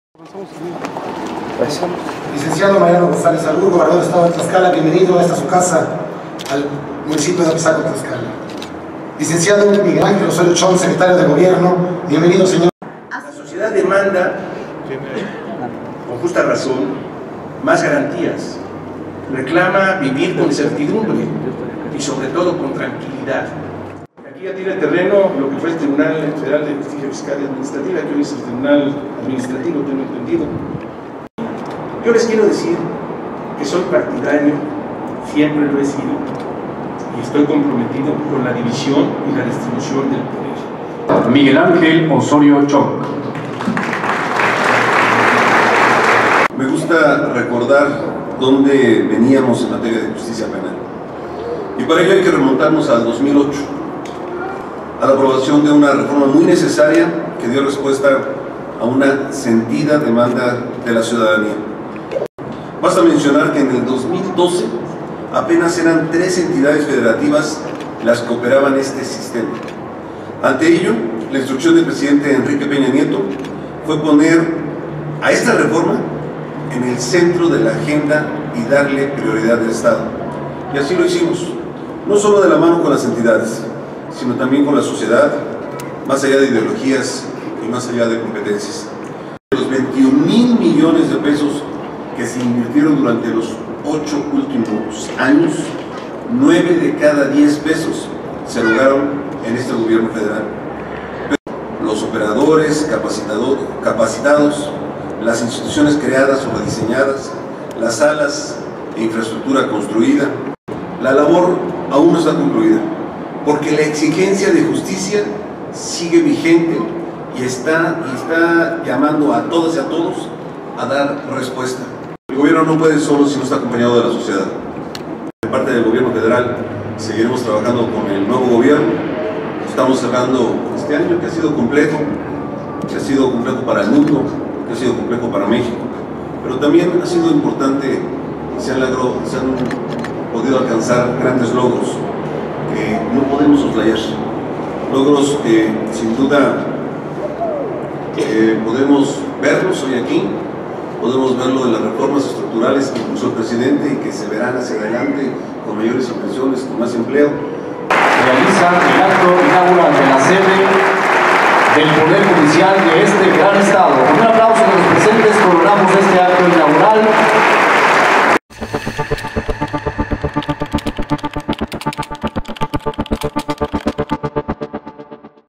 Reformas en Justicia Penal necesarias: Osorio Chong
"La exigencia de Justicia sigue vigente"

El secretario de gobernación Miguel Ángel Osorio Chong, declaró que desde la aprobación de reformas en materia de justicia, y su aplicación a nivel nacional no se ha escatimado en implementar el NSJP con 21 mil millones de pesos invertidos, nueve de cada 10 pesos fueron erogados por el gobierno federal.